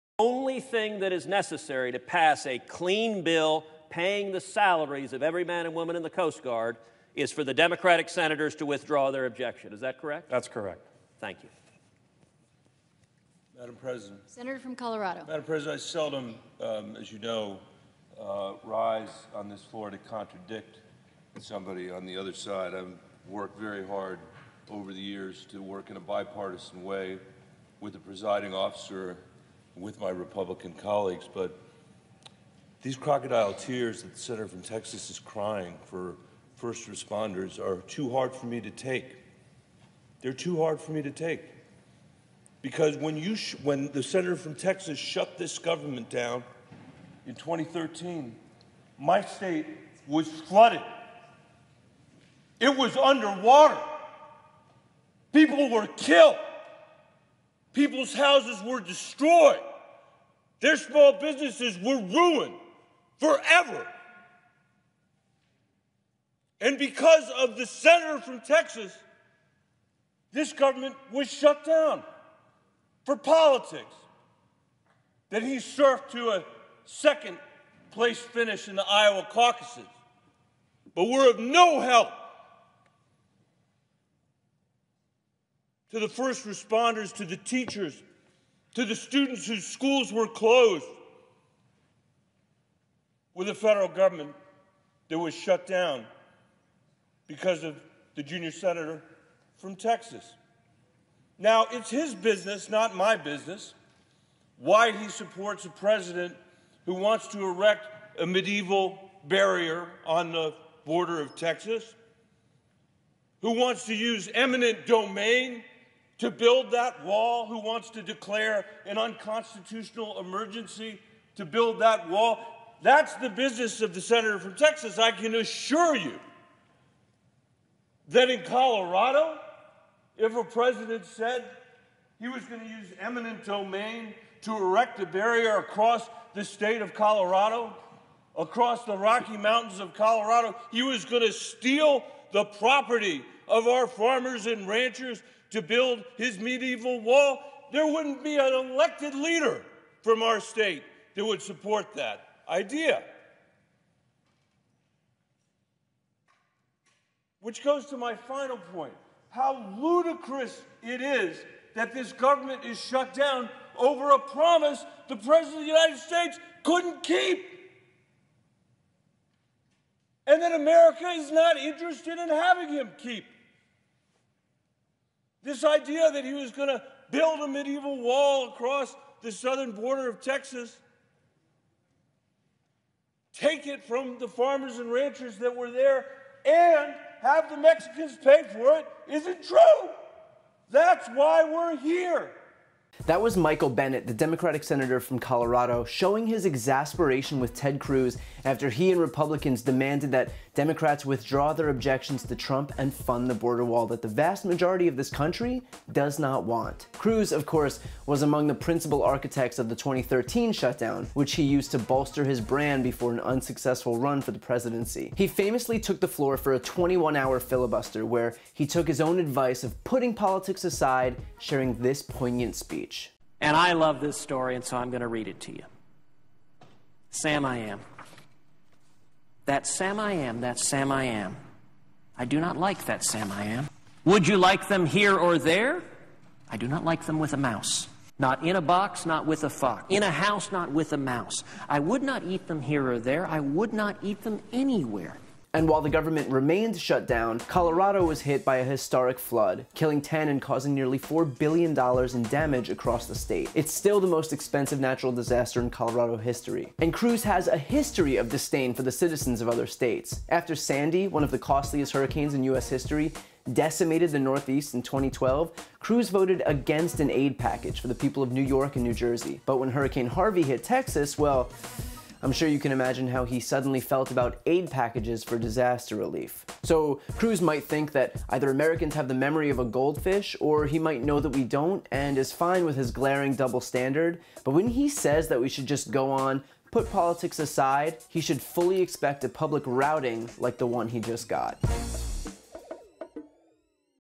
A Democrat just went viral with the best speech EVER on Senate floor
BREAKING: Democratic Senator Michael Bennet just went viral with the best speech EVER on the Senate floor. This is an absolute MUST-WATCH.